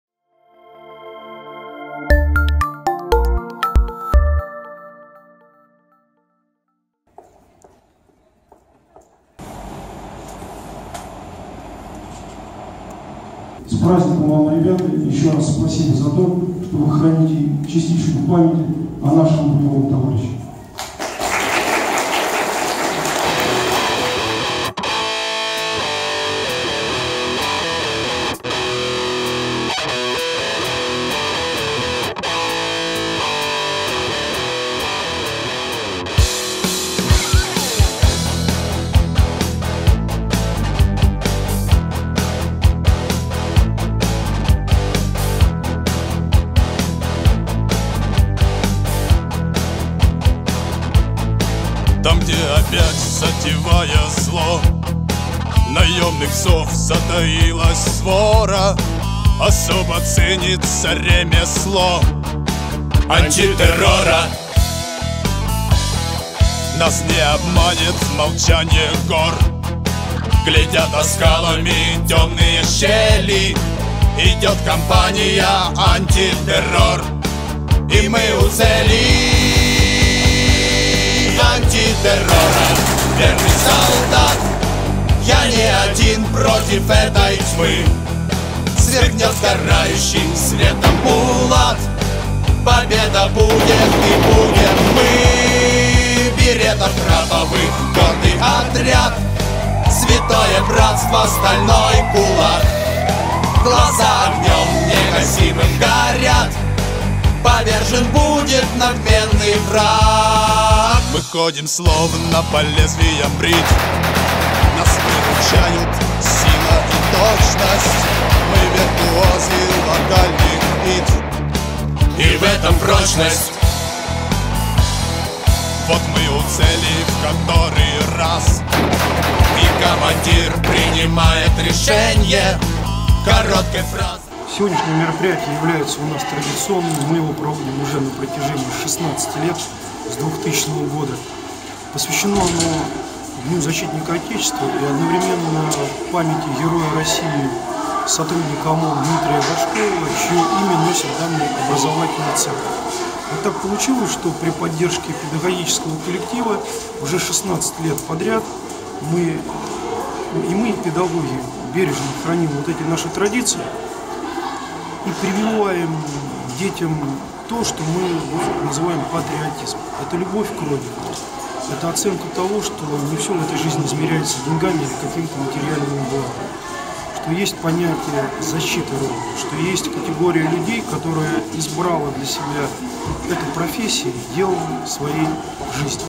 Тульский ОМОН провел «бой» во дворе Центра образования №1
Утром 19 февраля во дворе Центра образования №1 имени Героя России Дмитрия Горшкова в Туле прогремели автоматные очереди и взрыв - бойцы тульского Отряда Мобильного Особого Назначения (ОМОН) провели для учеников показательные выступления.
Показательным выступлениям предшествовали «уроки мужества», возложение цветов, небольшой торжественный концерт и мини-выставка вооружения. Ребята и девчонки с огромным интересом рассматривали защитные шлемы, пистолеты, автоматическое оружие, а также кольчужные перчатки для ножевого боя.
Напомним, Герой России, боец ОМОН Дмитрий Горшков погиб в 1999 году, исполняя служебные обязанности и спасая боевых товарищей на Северном Кавказе. 21 февраля Дмитрию Горшкову исполнилось бы 45 лет.